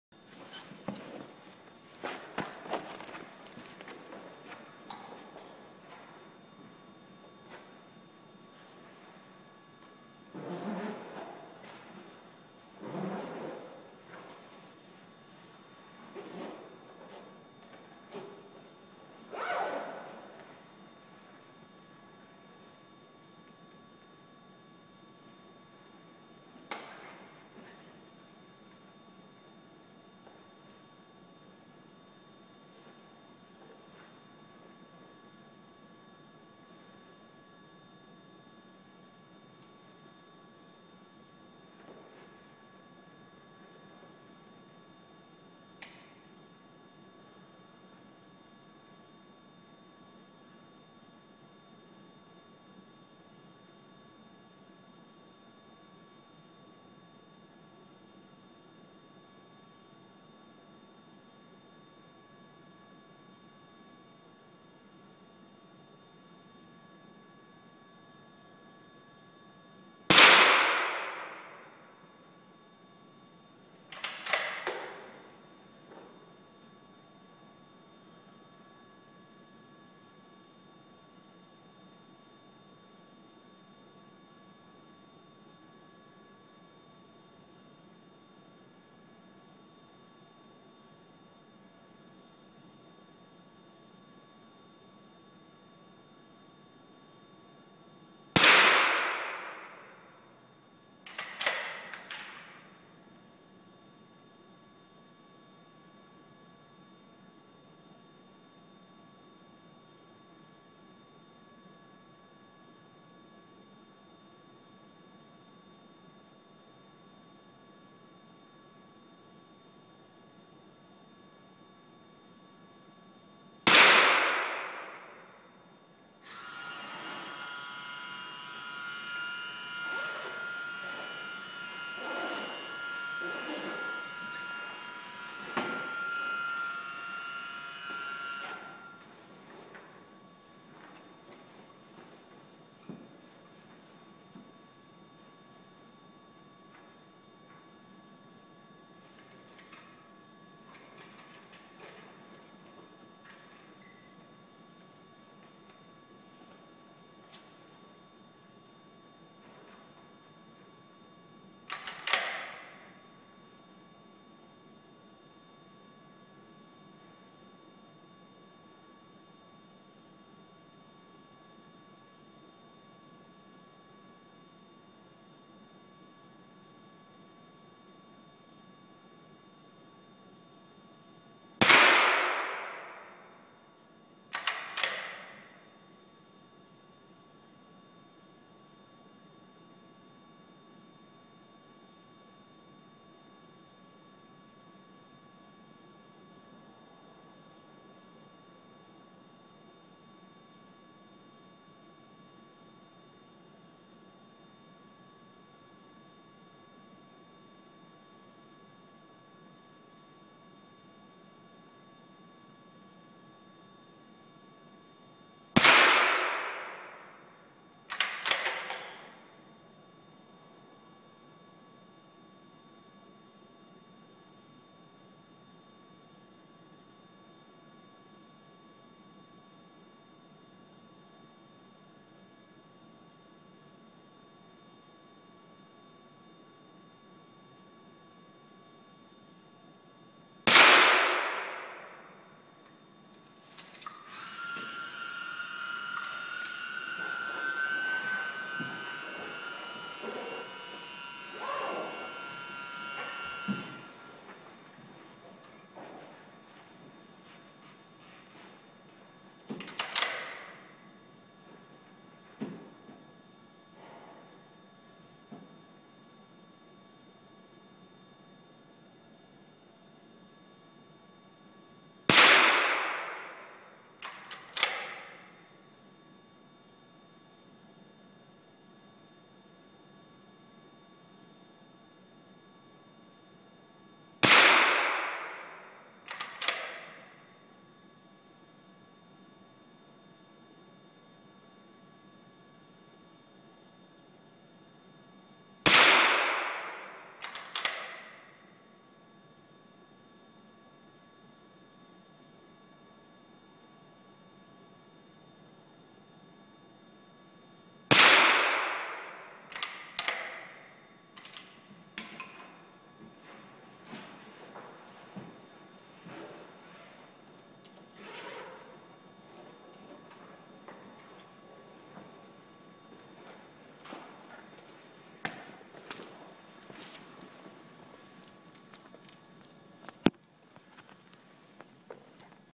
Trening - Grunnlag
Skyter Grunnlag på trening (uten klokke, brukt for lang tid) Gjort som en test med kamera for å se om det funket.